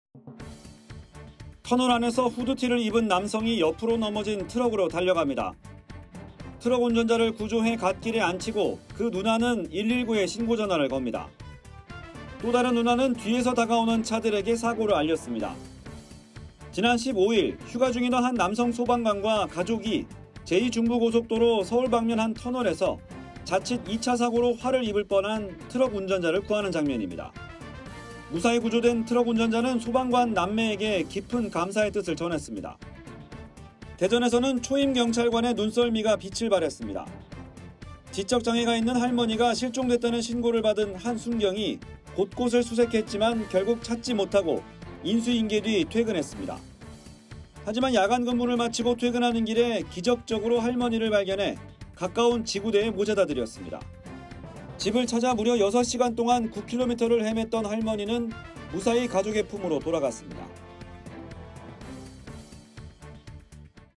[자막뉴스] 넘어진 트럭에 달려가 역할 분담 '척척!' 소방관 남매의 팀워크| 이슈픽
사실 보이지 않을 뿐, 우리 곁에는 묵묵히 자기를 희생하는 소방관과 경찰관들이 아주 많죠.

시민을 지키는 그들의 노력은 퇴근 후라고, 쉬는 날이라고 멈추지 않습니다.

요 며칠 절로 박수를 쳐주고 싶은 우리 소방관과 경찰관의 조용한 헌신이 영상으로 올라와 화제가 됐습니다.